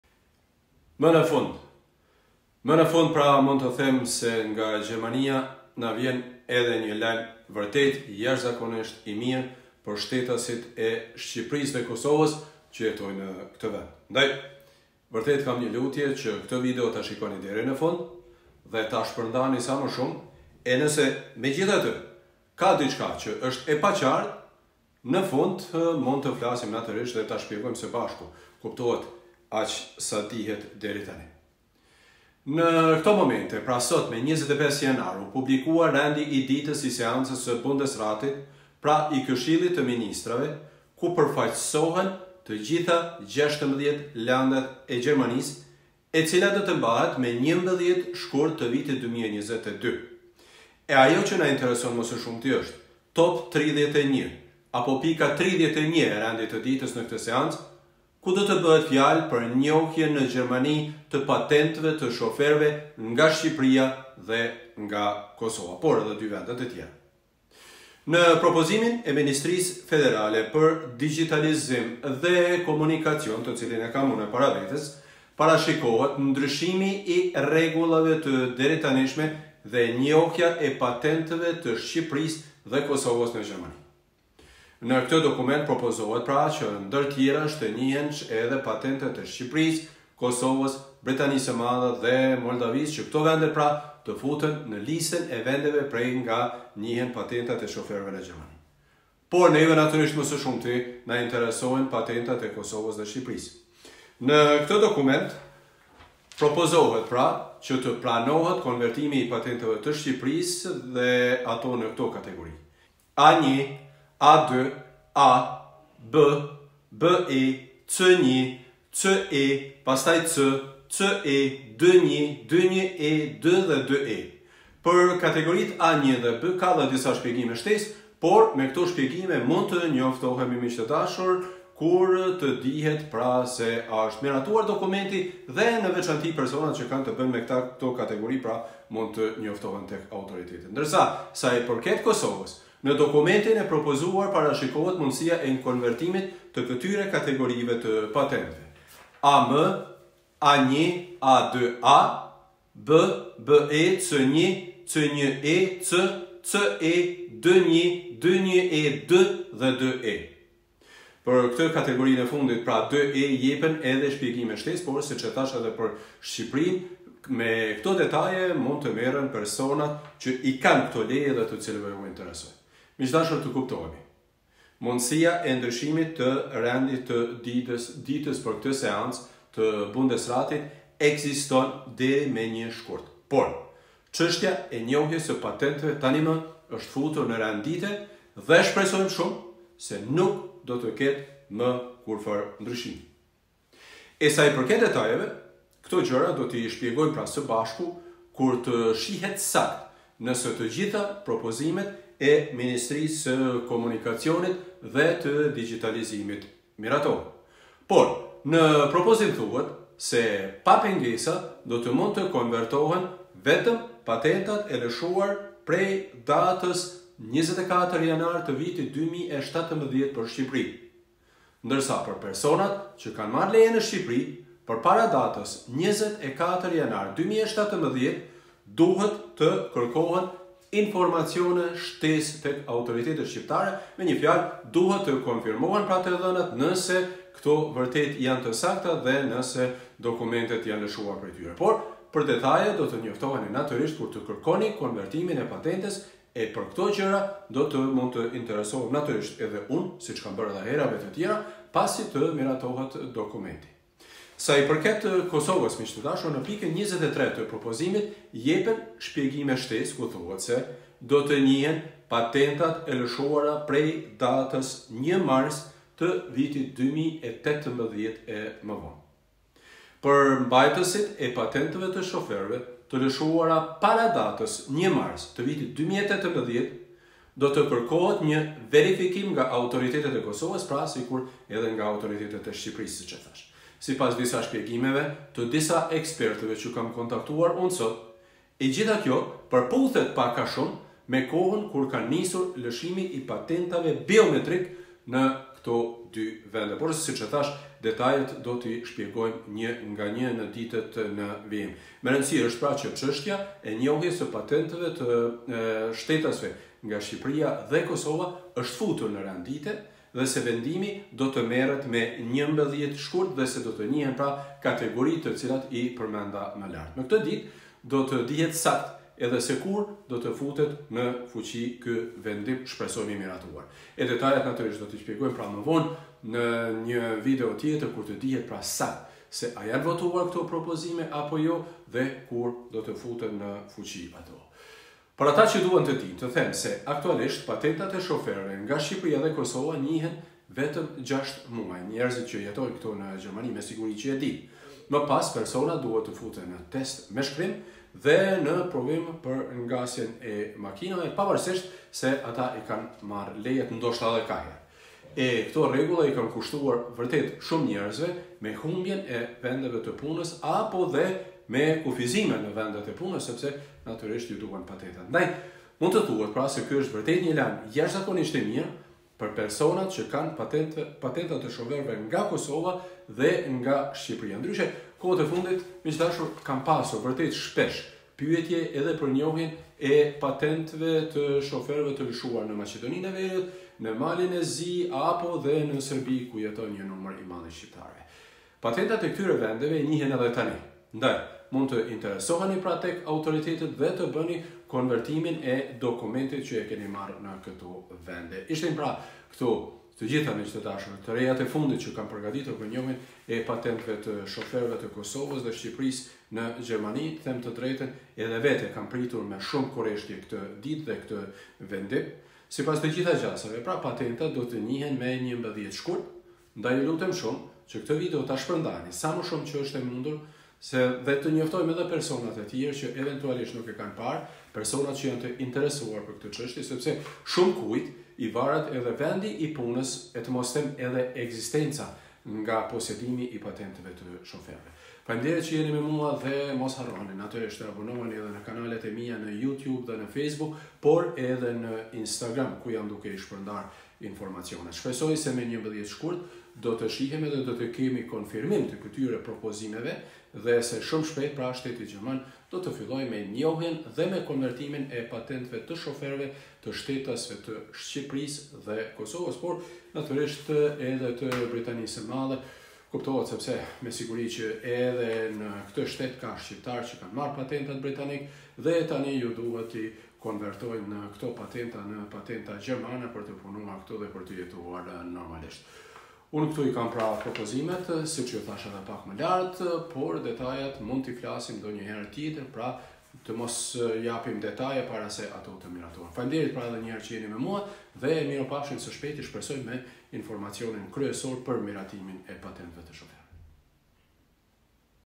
Së shpejti: Njohja e patentave të shoferëve në Gjermani! | Bahri Cani
Në seancën e ardhshme të Bundesratit, që mbahet me 11 shkurt 2022,  do të trajtohet edhe çështja e njohjes së patentëve të shoferëve nga Shqipëria dhe Kosova. Abonohu dhe shpërndaje...